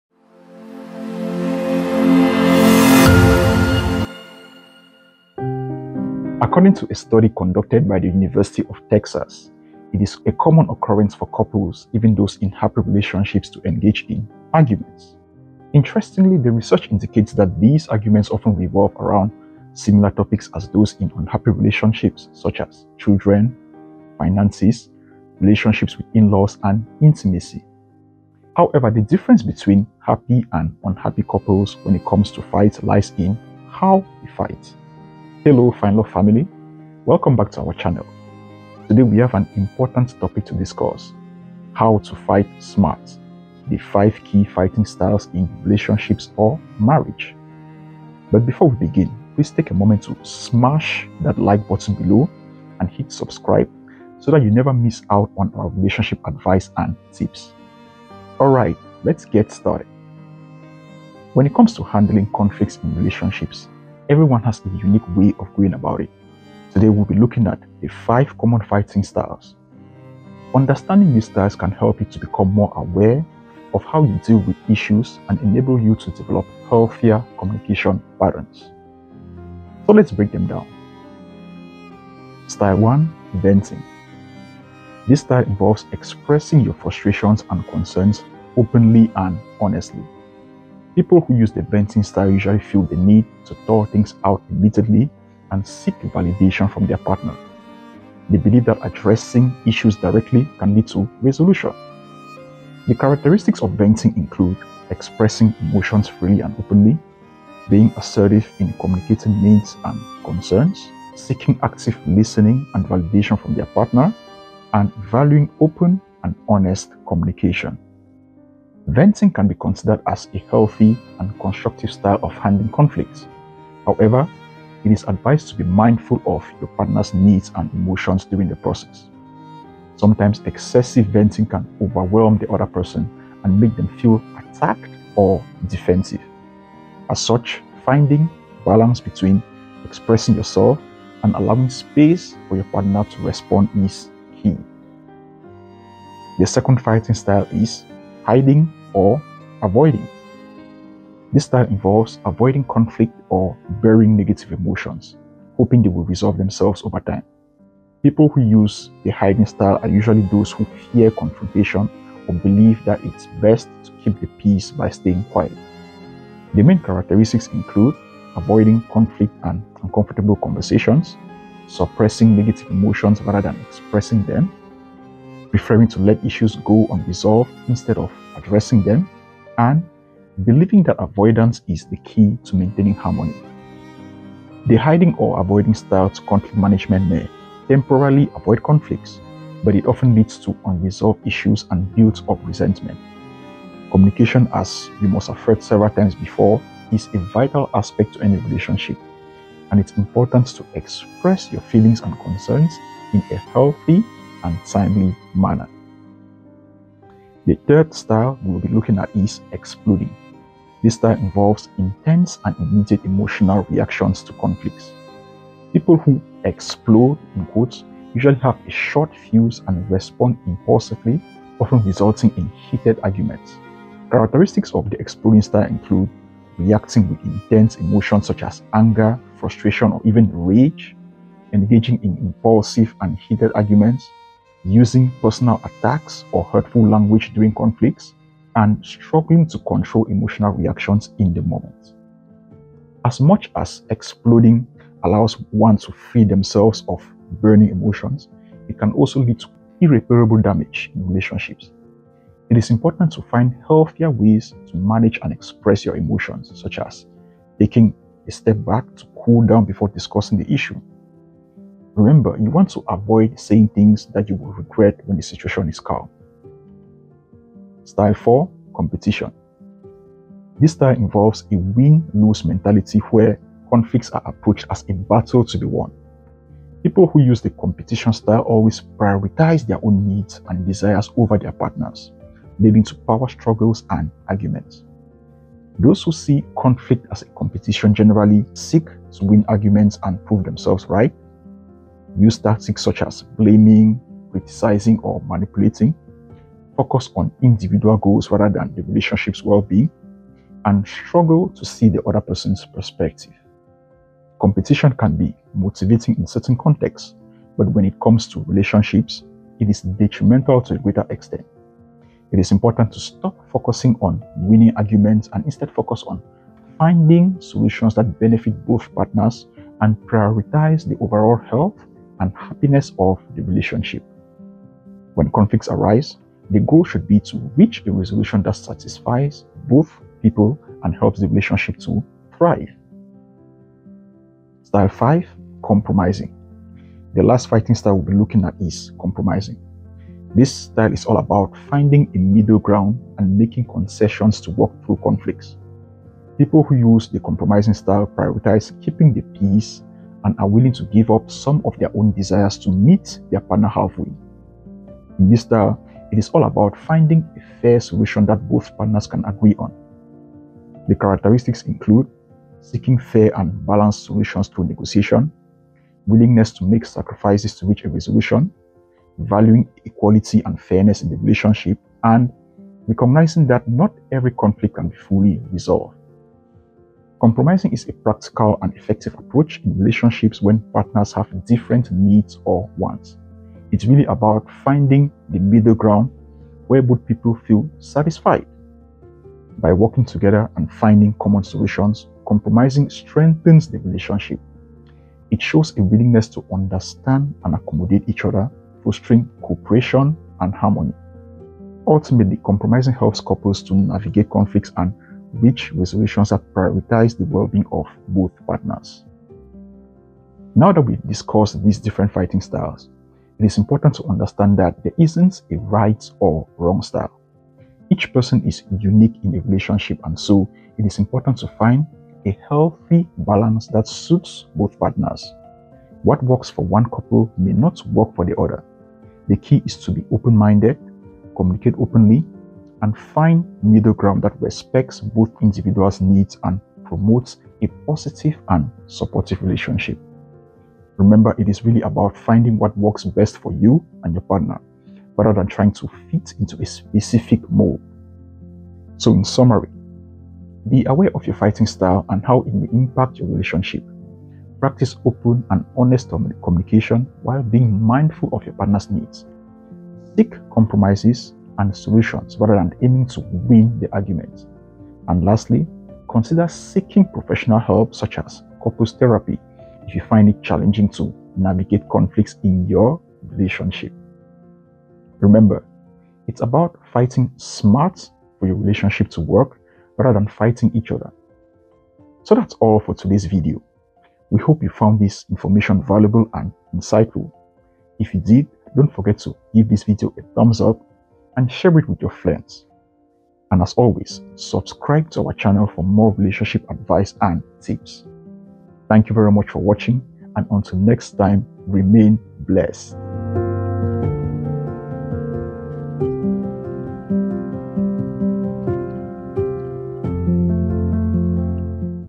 How to FIGHT SMART and NOT HARD in Relationships | How to NOT ARGUE in Marriage
It's time to avoid the long and draining arguments. Learn how to fight smart and not hard in relationships or marriage. Unlock the Secrets of Relationship Strategy: Mastering the Art of Fighting Smart! Discover how to navigate disagreements with finesse, communication, and understanding. Transform conflicts into opportunities for growth and deeper connection. Join us as we unveil the keys to preserving harmony while navigating the complexities of love and partnership.